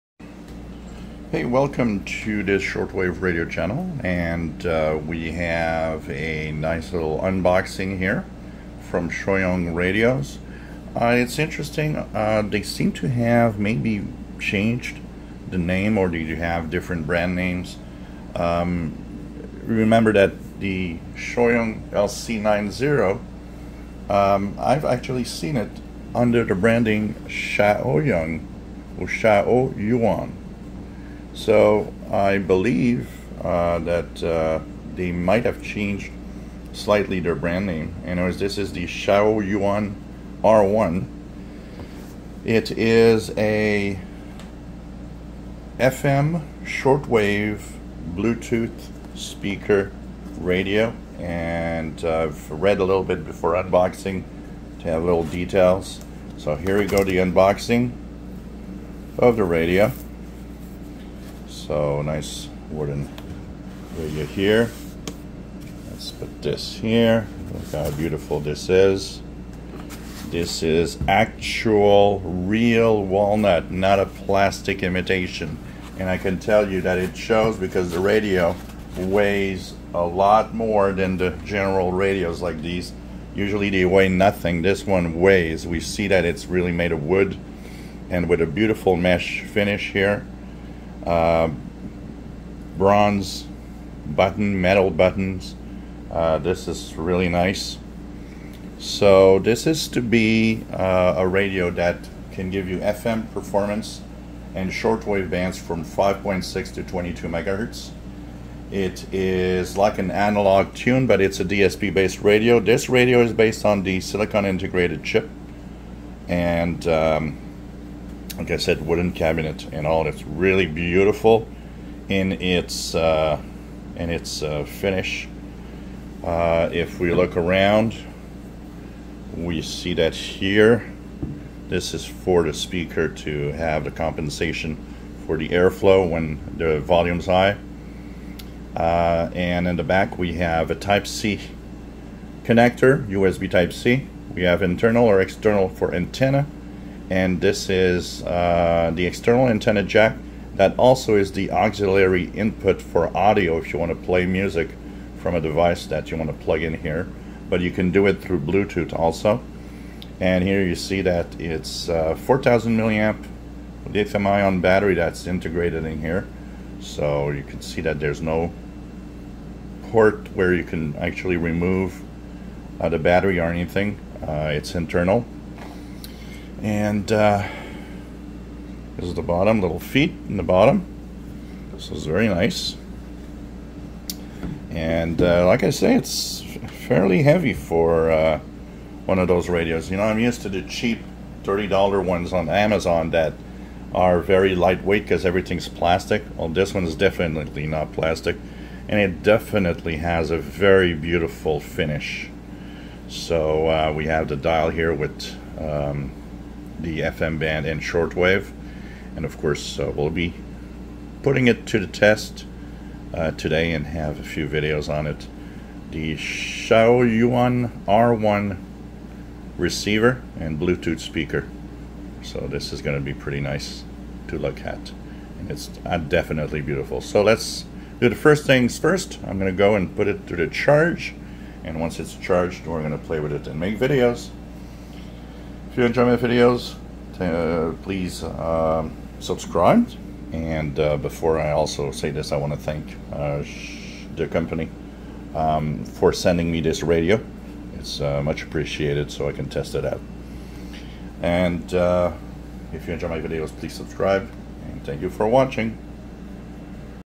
Unboxing the ChaoYuan R1 FM Shortwave receiver Bluetooth Speaker
Interesting radio very beautiful , real Wooden Walnut cabinet .  not shown here includes a wire with connector for antenna and charging cable